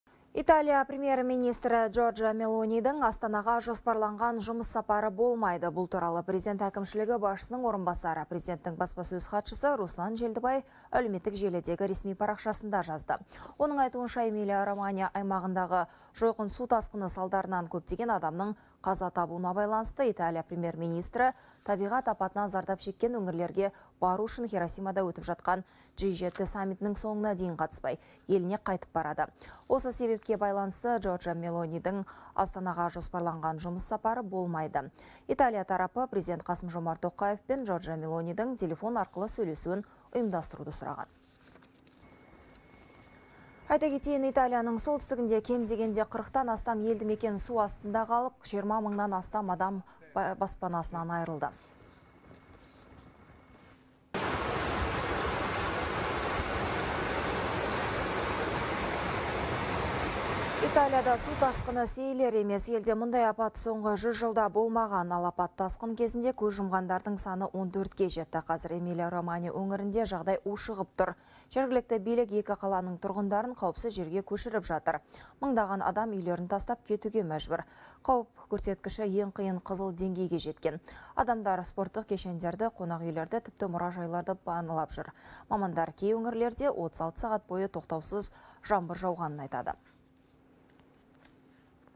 Италия премьері Астанаға келмейтін болды
Италия Премьер-министрі Джорджа Мелонидің Астанаға жоспарланған жұмыс сапары болмайды.